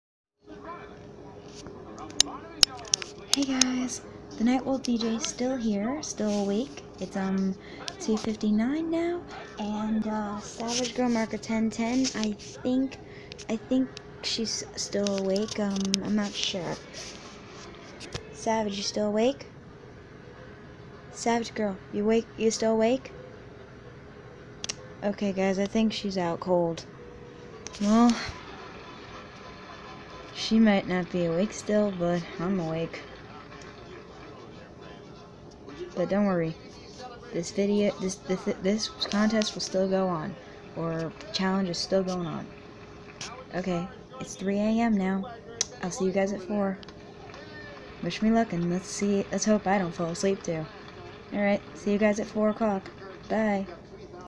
3:00 AM
Savagegirlmarker1010 is still awake and so am I but I don't know how much longer she will be awake. Don't worry about me though I'm still wide awake and I will get to 6:00 AM